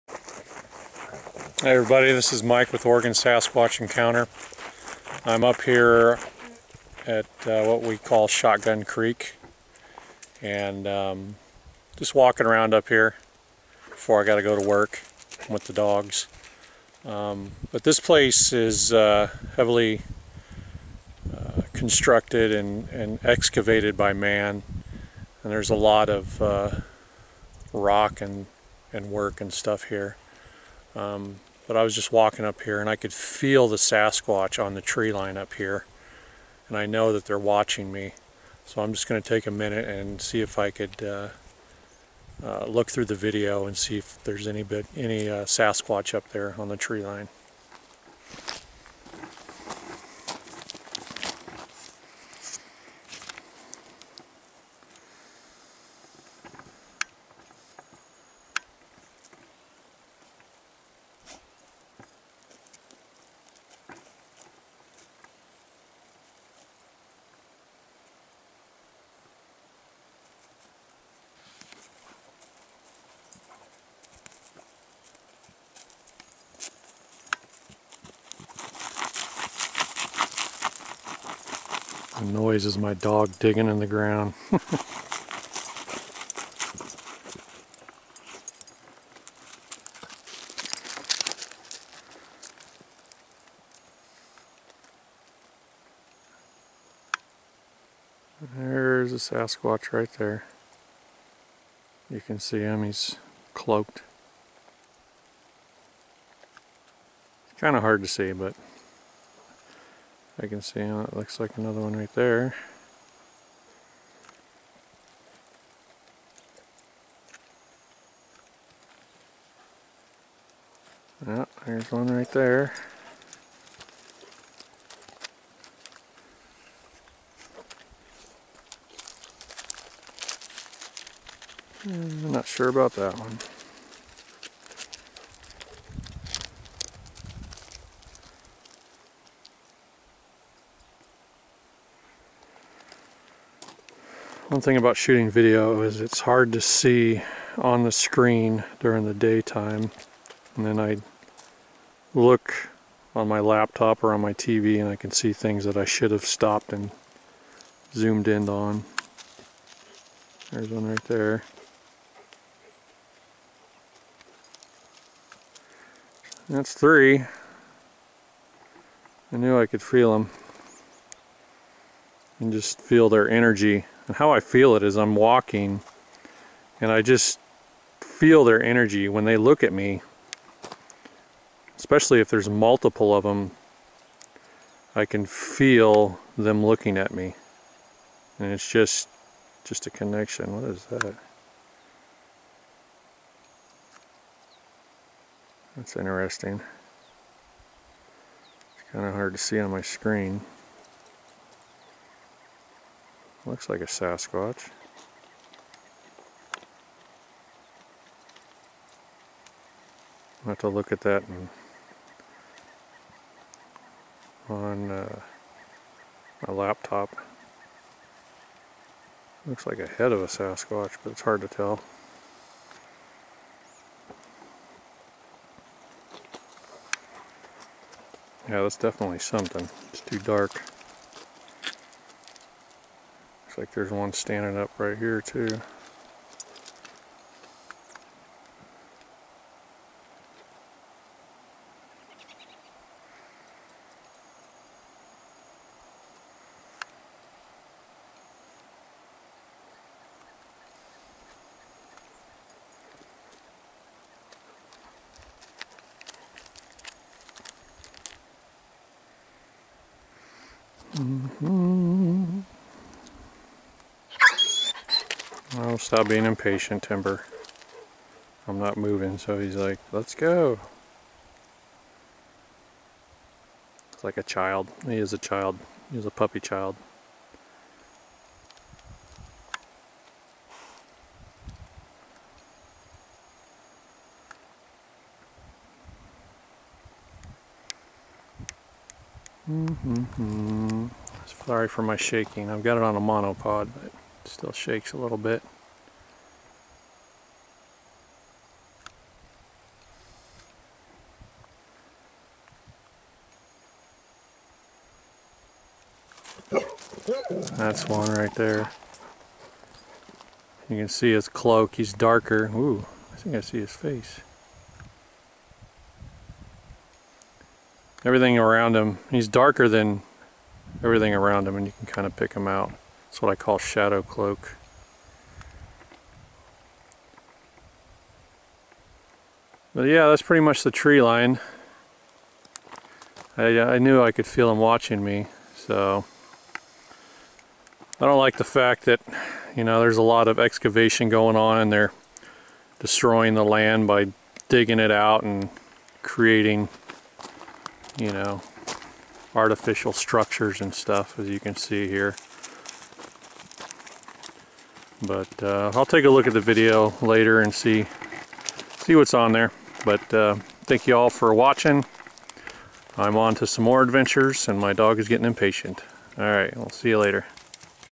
Feeling the Sasquatch watching on the treeline. I was right.
I felt the Sasquatch watching me on the treeline. I was right. There are many up by the treeline watching me and my dogs.